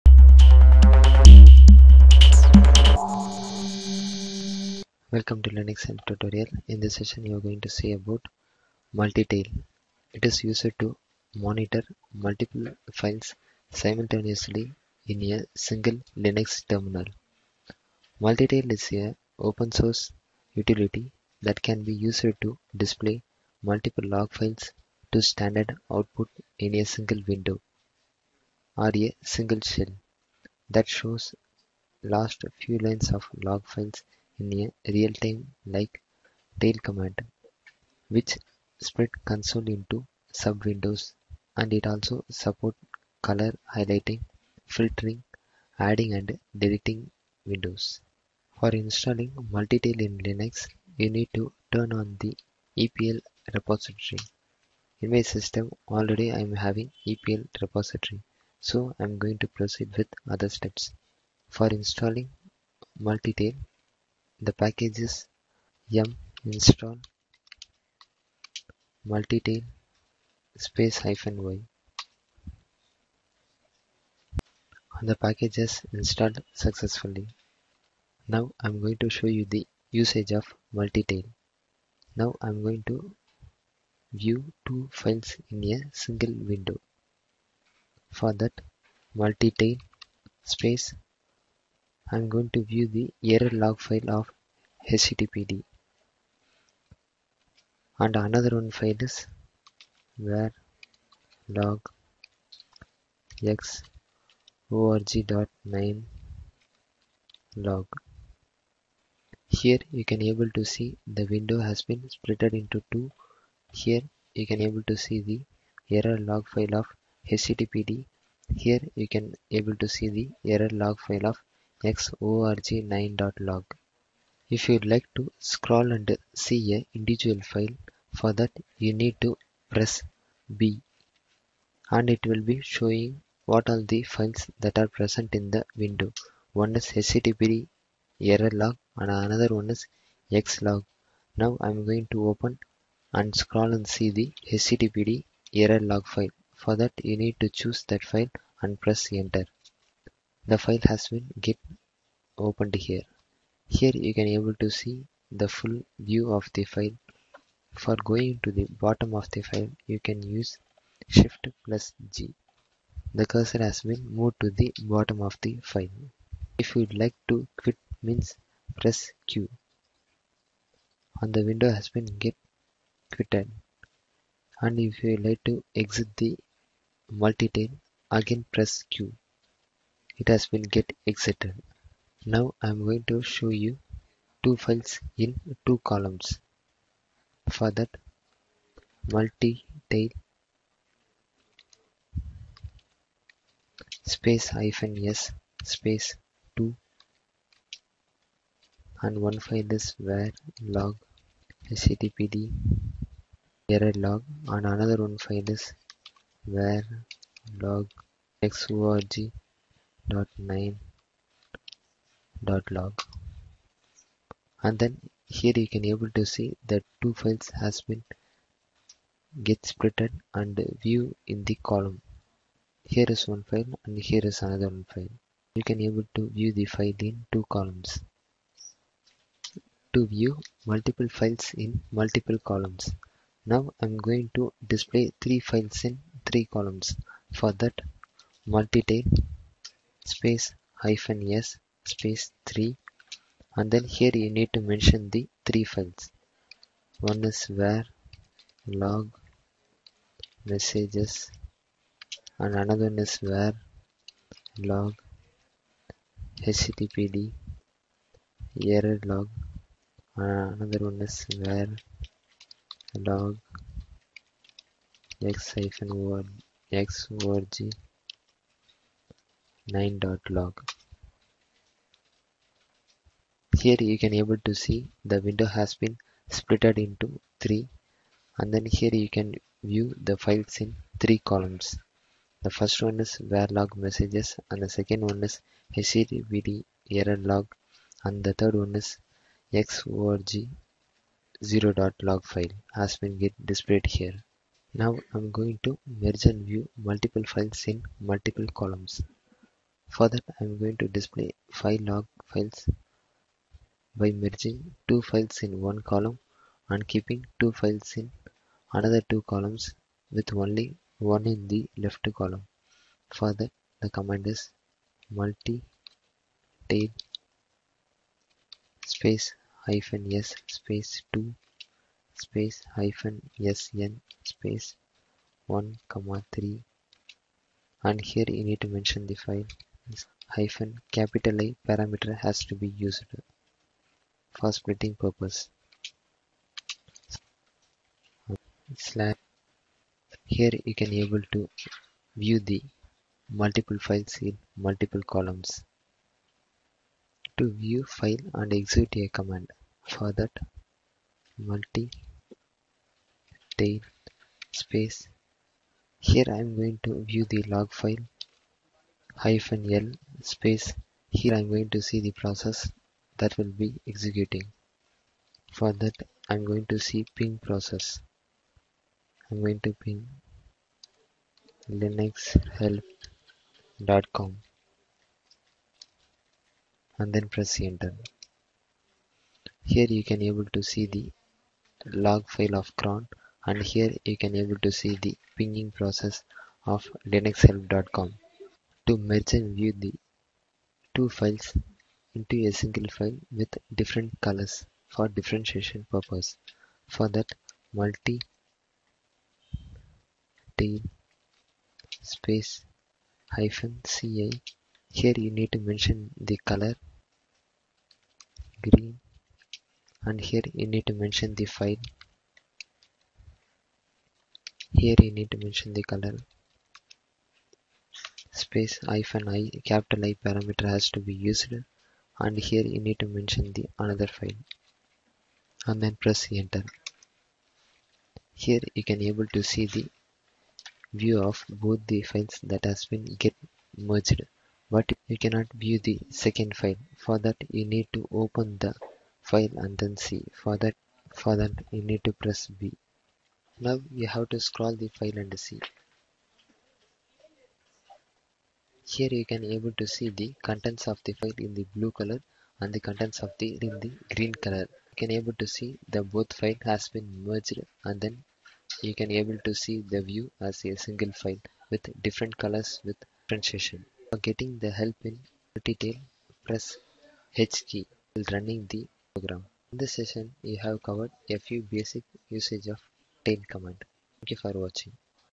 How to Install of MultiTail - Monitor Multiple Files Simultaneously in a Single Linux Terminal
This video illustrates the Installation process of MultiTail and its features. It is a program used to display multiple log files for standard output.